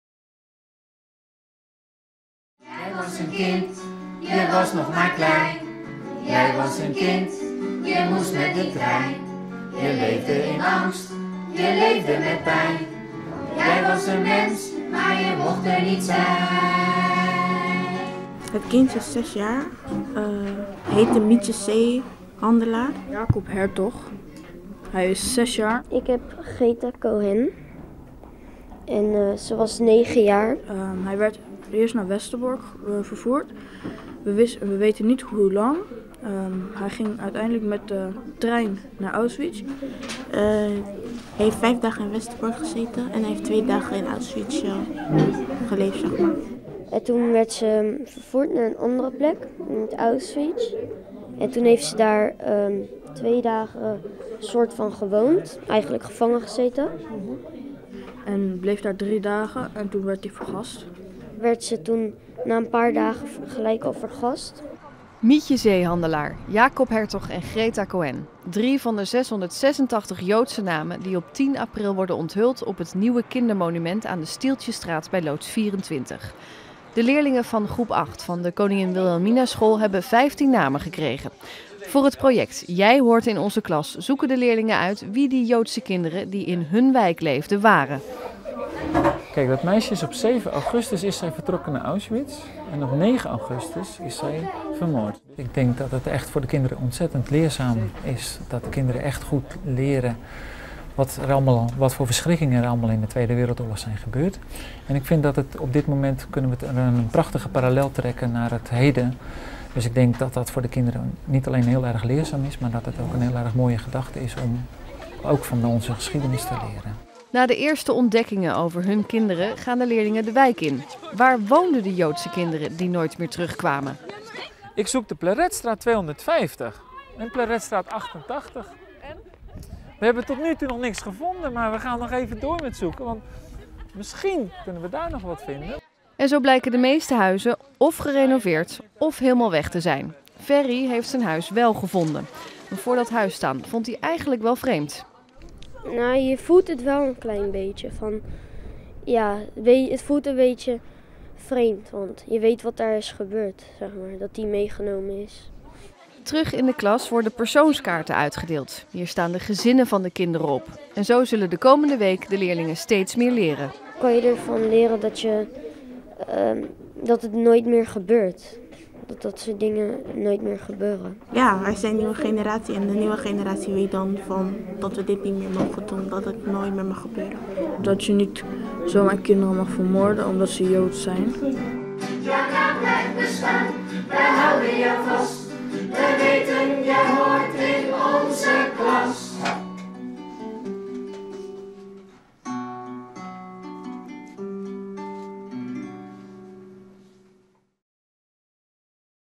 Rotterdamse leerlingen leren over Joodse kinderen
686 Joodse kinderen werden tijdens de Tweede Wereldoorlog vanuit Loods 24 aan de Stieltjesstraat in Rotterdam op transport gezet naar concentratiekampen. Voor deze kinderen wordt op 10 april een monument onthuld.

   Lees meer: Rotterdamse leerlingen leren over Joodse kinderen